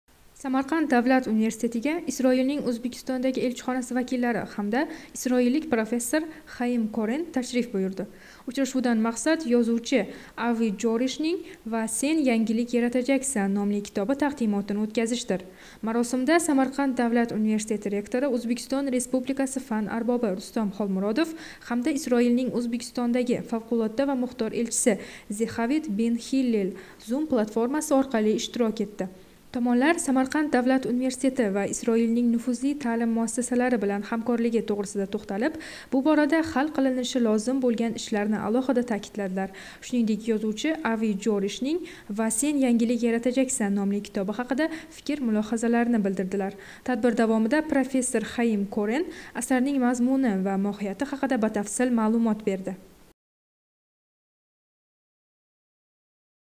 Samarqand davlat universitetida “Va sen yangilik yaratajaksan” nomli kitob taqdimoti o‘tkazildi…
Samarqand davlat universitetining ijtimoiy tarmoqlardagi sahifalariga obuna bo'ling!